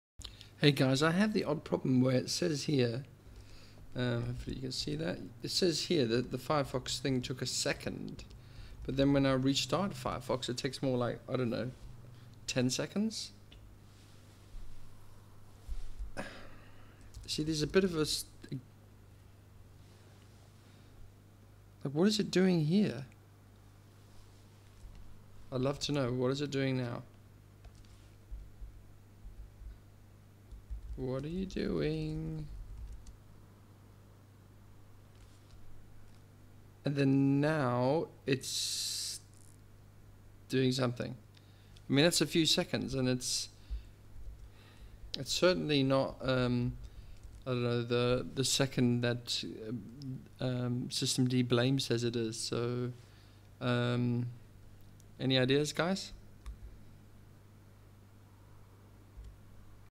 How do I measure how fast a process is spawning?
systemd-blame seems way off the mark.

I'm using 215-17+deb8u2 from Debian Jessie here